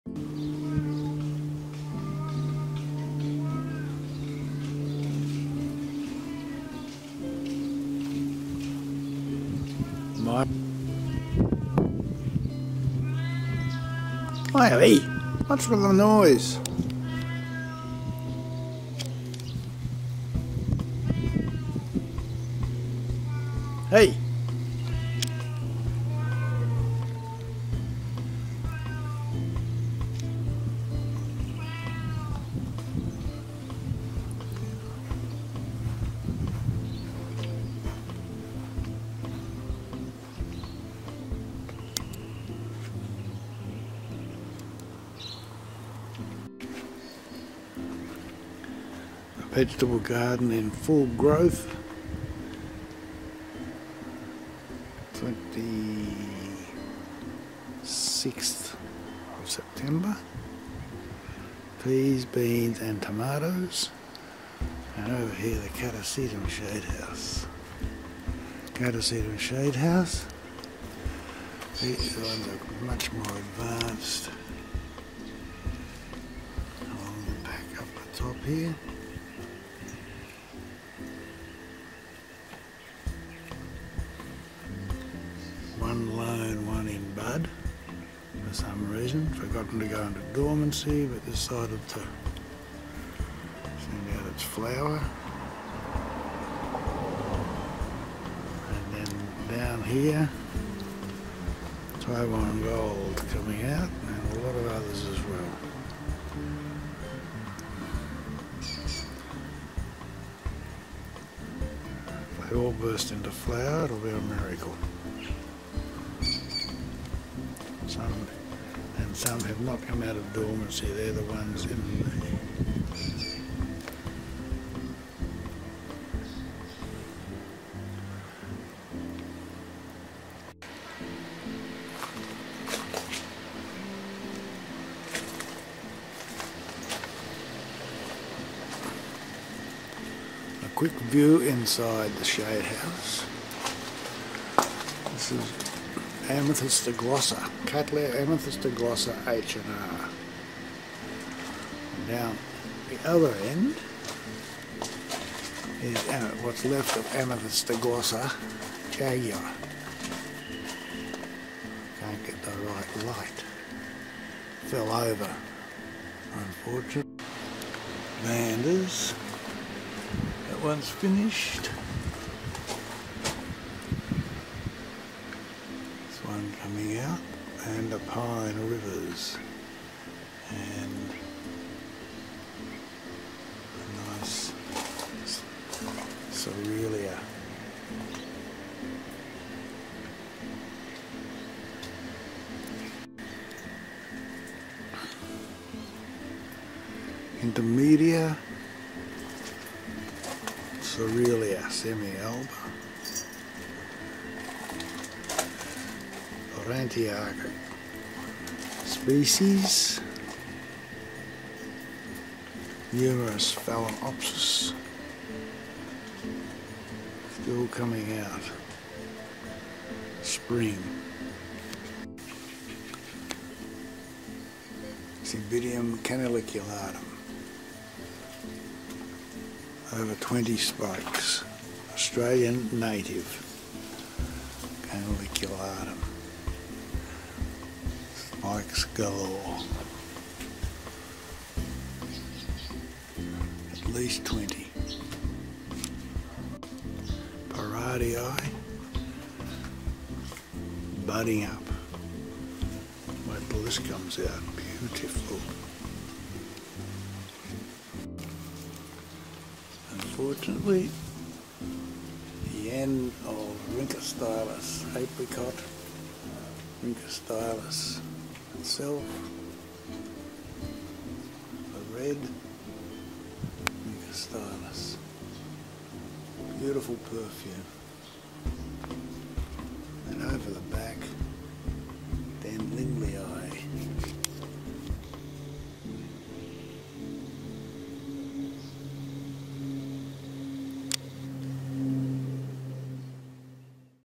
Orchids Down Under
An occasional ramble about orchid growing in SE Queensland, Australia